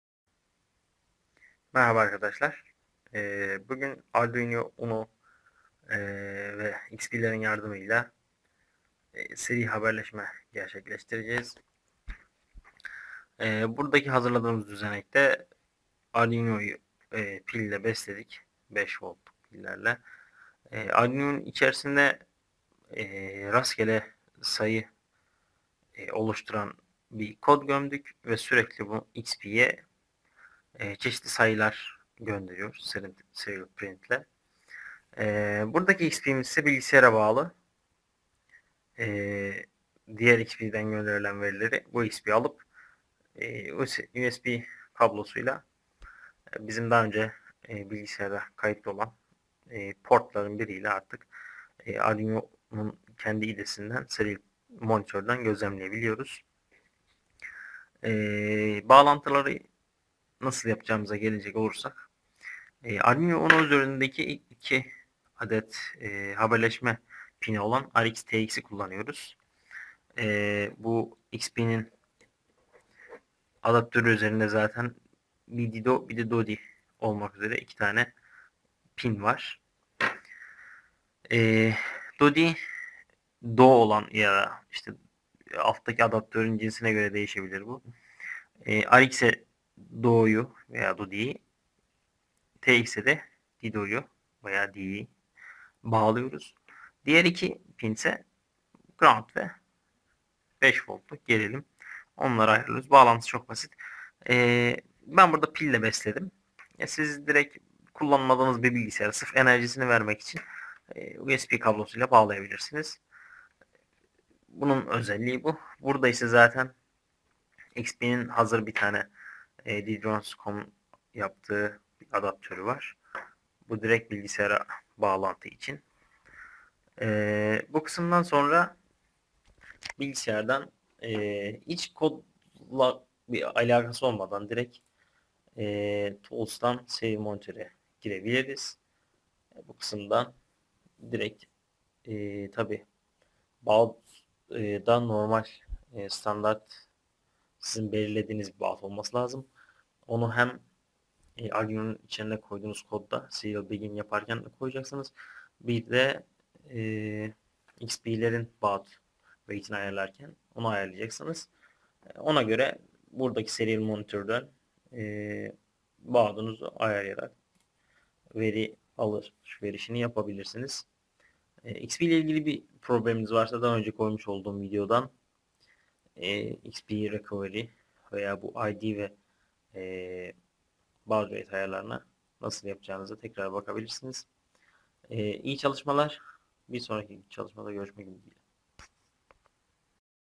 Xbee ile Seri Haberleşme
Uygulamalarınızı uzaktan kontrol etmek ister miydiniz ?
Eğer cevabınız evetse bu çalışmamız tam size göre demektir.

Bu çalışmamızda Xbee kullanarak Arduino içerisine gömdüğümüz rastgele sayı döndüren kodumuzun sayısal çıktılarını bilgisayarımıza bağlı olan bir diğer Xbee sayesinde okumaya çalışacağız.

Gerekli olan malzemeler:

2 adet Xbee
2 adet Xbee adaptörü
Xbee adaptörü-Pc bağlantı kablosu
Arduino beslemek için 5V kaynak
Bağlantı kabloları

İlk olarak yapmamız gereken bize rastgele sayı üretecek olan kodu hazırlayıp Arduino'ya gömmek.
Bu kısımda Serial.begin(); kısmındaki vereceğimiz baud rate değerine dikkat etmemiz gerekiyor. Bu sayının Xbeelerimizin baud rate'leri ile aynı olması gerekiyor.

Kodumuza gelecek olursak:

//M.Furkan Bahat
//Rastgele Sayı üretmek

int Rastgele;

  Serial.begin(9600);
  randomSeed(analogRead(0));

void loop()
  Rastgele = random(1000);
  Serial.println(Rastgele);
  delay(1000);

Kodumuzu Arduino'ya gömdükten sonra yapmamız gereken Xbee-Arduino bağlantısını yapıp bu ikiliyi Serial Monitorden gözlem yapacağımız bilgisayarımızdan yalıtılmış bir hale getirmek olacak.

Arduino'yu nasıl besleyeceğiniz size kalmış, ana bilgisayardan ayrı olması yeterli.

Diğer Xbee'ye gelecek olursak, o arkadaşı da adaptörü vasıtasıyla bilgisayarımızın USB girişine bağlayıp tanıtıyoruz.

Bütün işlemleri tamamladıktan sonra tek yapmamız gereken Arduino IDE'yi açıp seri ekrandan verileri almak olacaktır. Herhangi bir sıkıntı yaptı iseniz Baud rate'inizi, Xbee'lerinizin çalışabilirliklerini ve bağlantılarınızı gözden geçirmelisiniz.